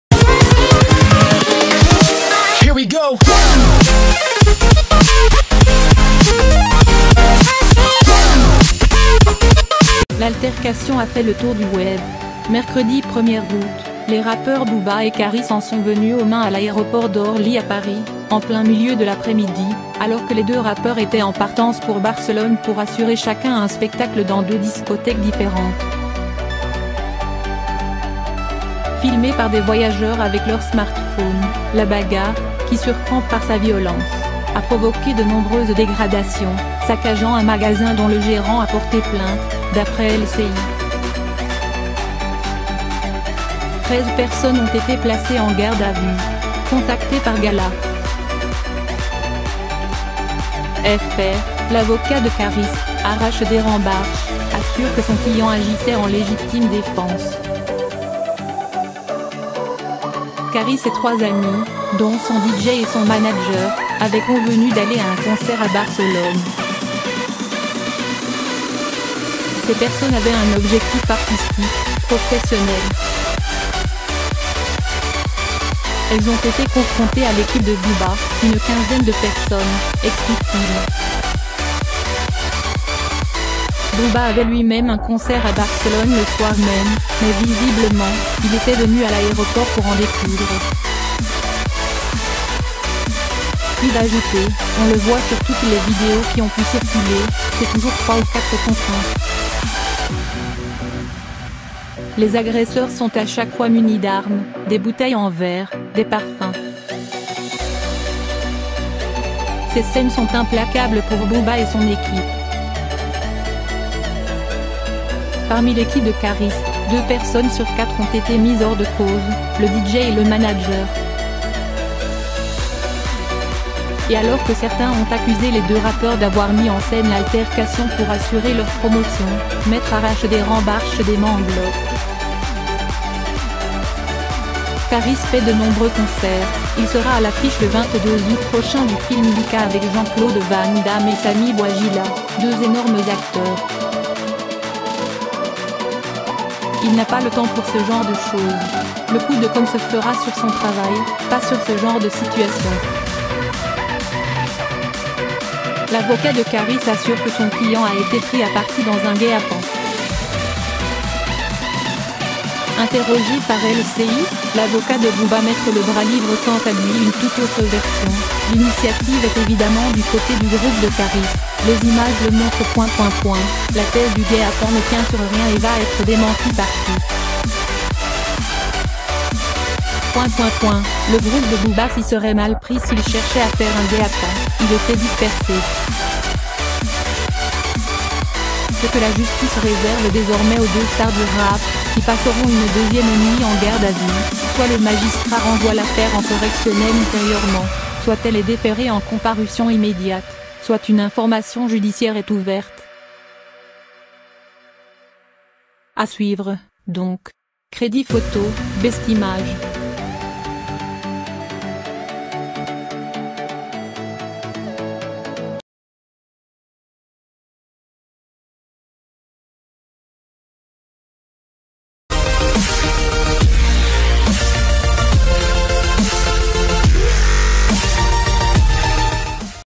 EXCLU – Bagarre à Orly : « Booba est venu à l’aéroport pour en découdre »selon l'avocat de Kaaris, A
L'altercation a fait le tour du web. Mercredi 1er août, les rappeurs Booba et Kaaris en sont venus aux mains à l'aéroport d'Orly à Paris, en plein milieu de l'après-midi, alors que les deux rappeurs étaient en partance pour Barcelone pour assurer chacun un spectacle dans deux discothèques différent...